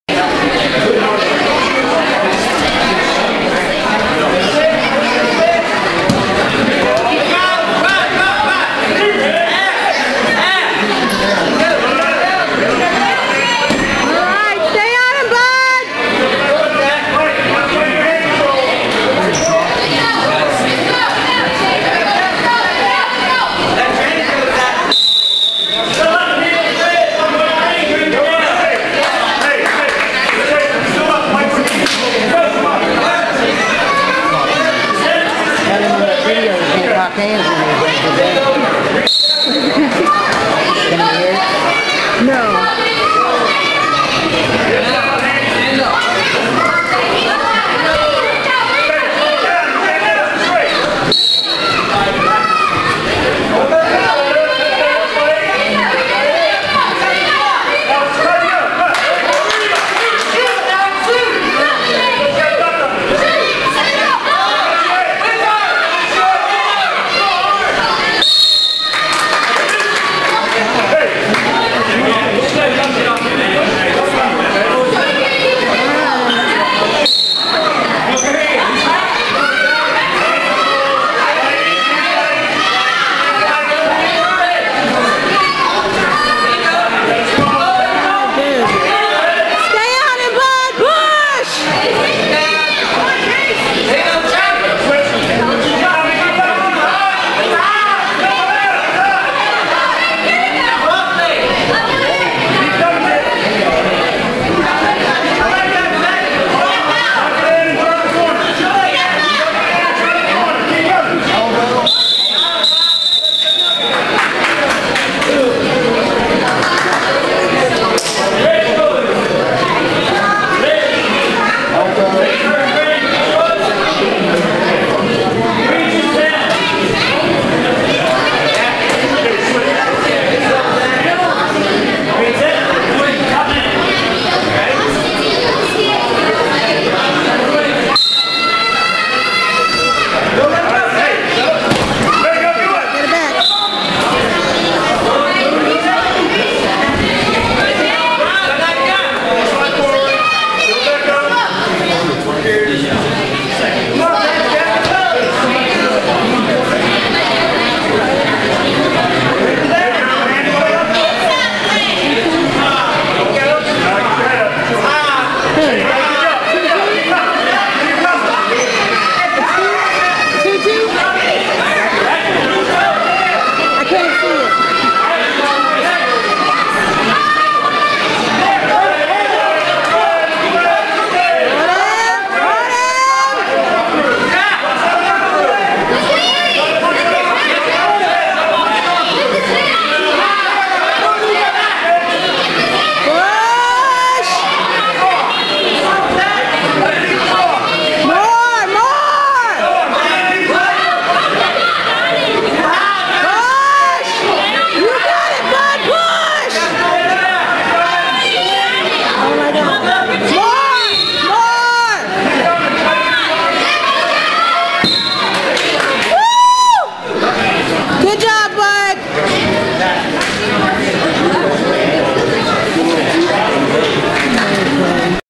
Powhatan Fall Classic 11/25/2012
Zach competing in the Powhatan Fall Classic on 11/25/2012. Powhatan, VA.